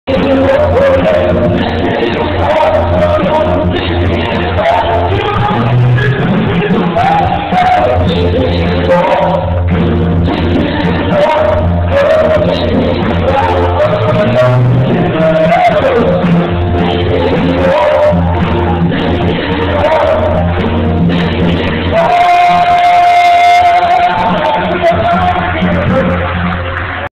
Pentecostal Church Worship ~ Point of Grace, Madison, IN
Church Service at Point of Grace in Madison, IN. My wife Stacy leading the Music along with Point of Grace singers